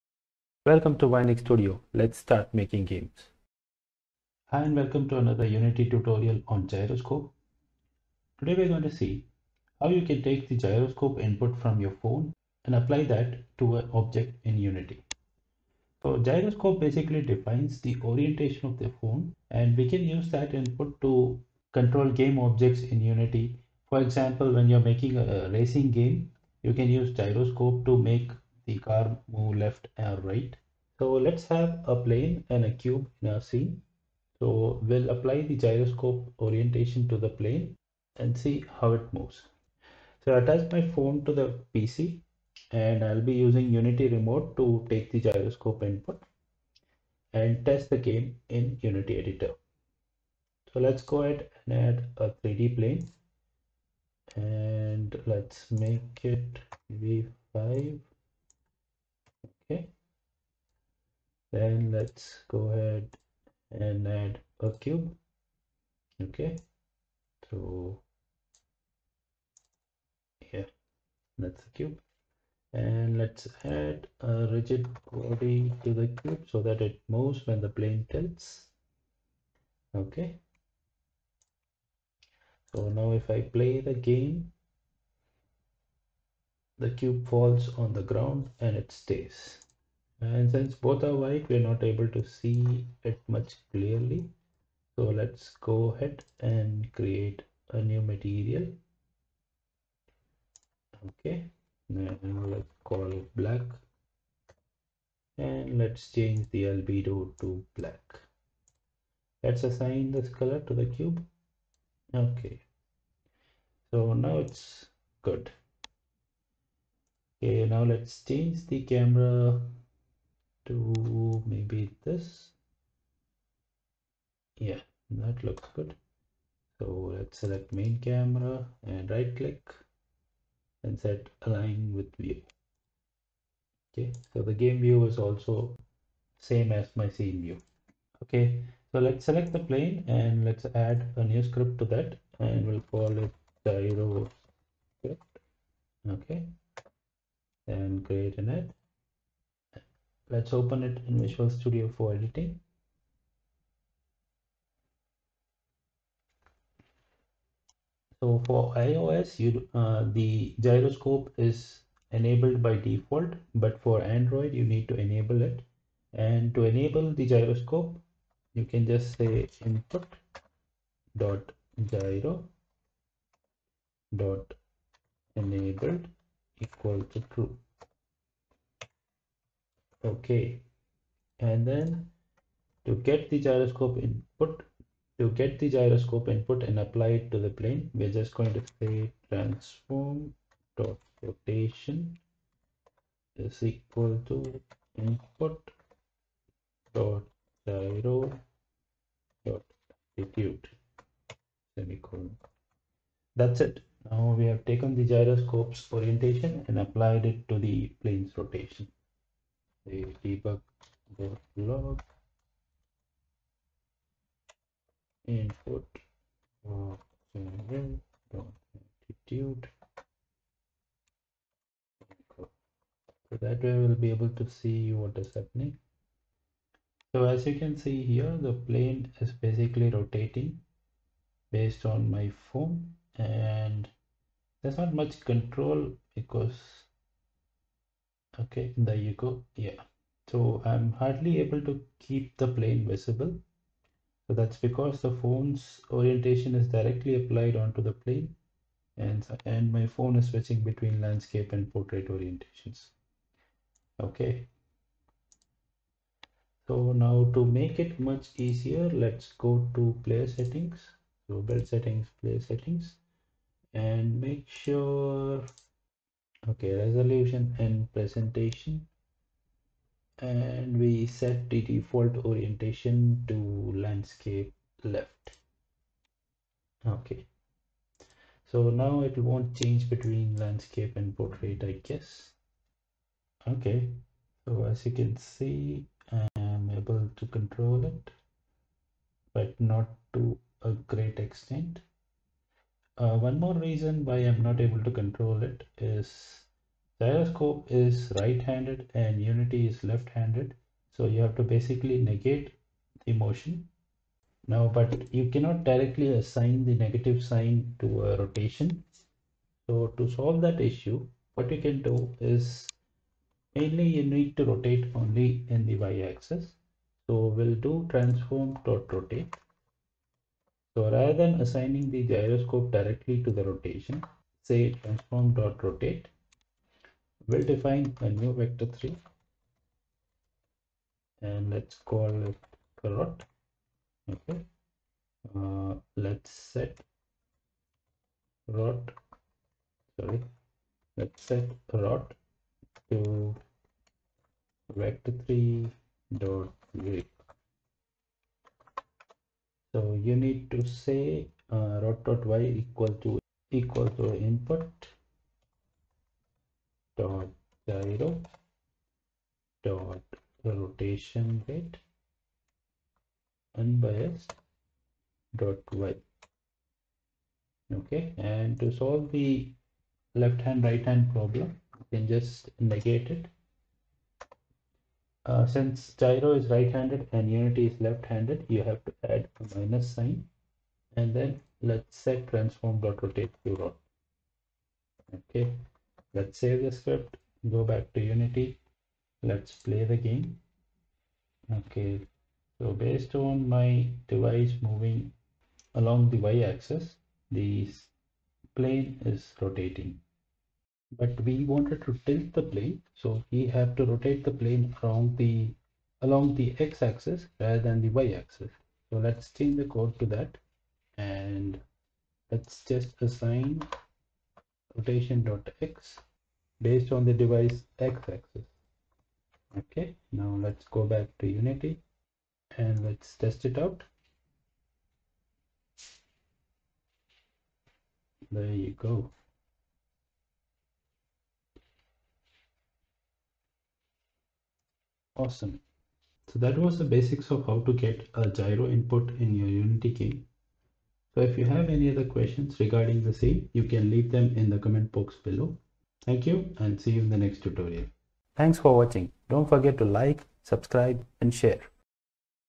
Unity Gyroscope tutorial | use phones Gyroscope as Input
Learn how to use the phone's Gyroscope as input for your game in Unity. You can get the code sample from